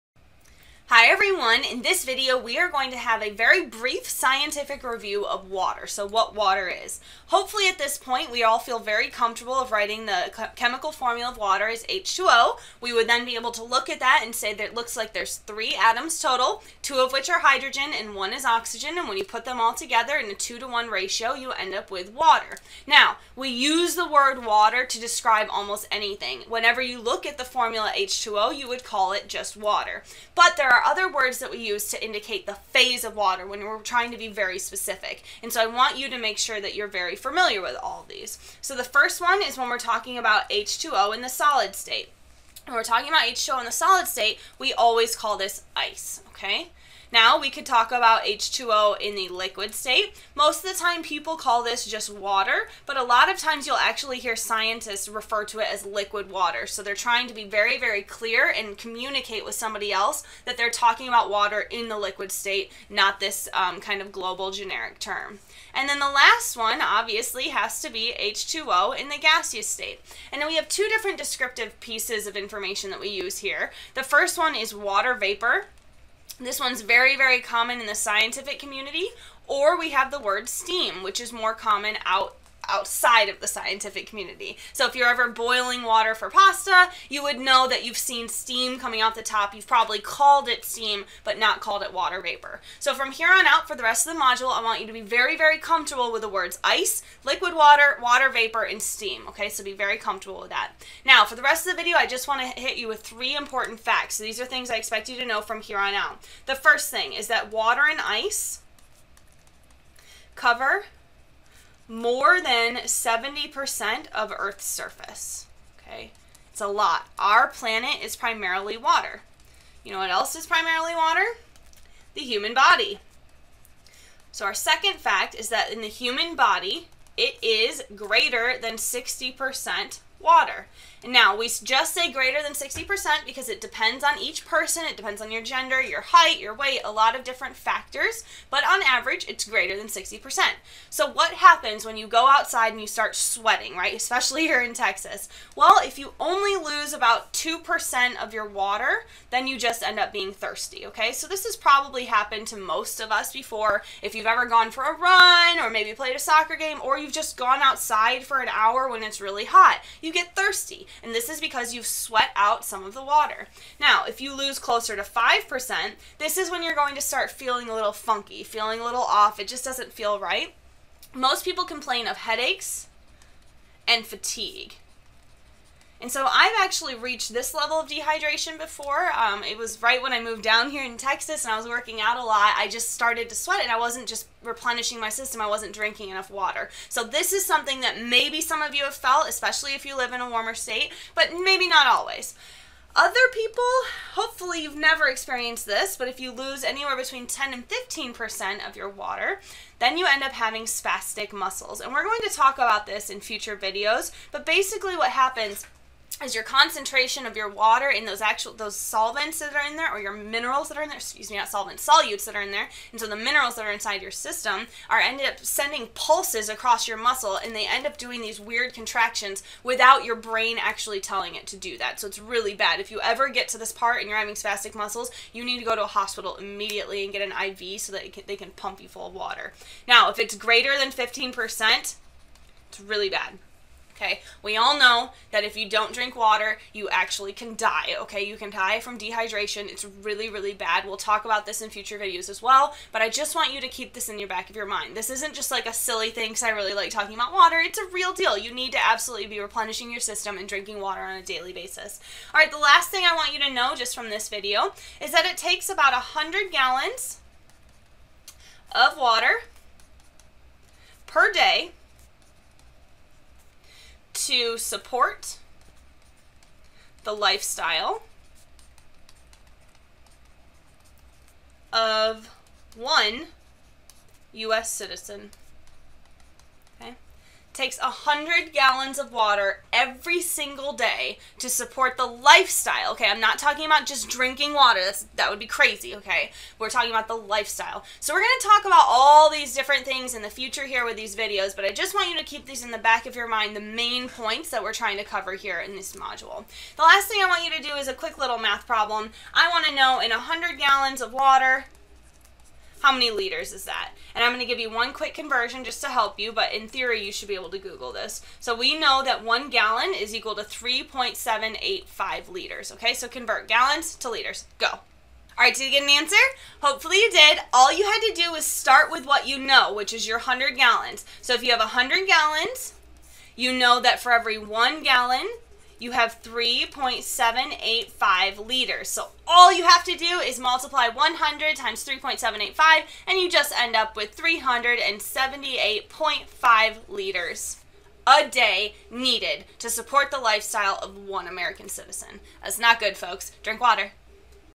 The Water We Drink
A teaching video on The Water We Drink used in 'The Water We Drink' module at The University of Texas at Austin.